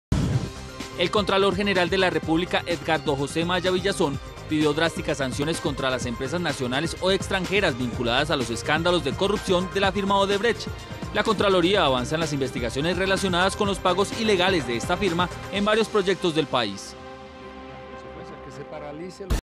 Contralor pide drásticas sanciones contra empresas vinculadas a caso Odebrecht
Vía: TeleAntioquíaNoticias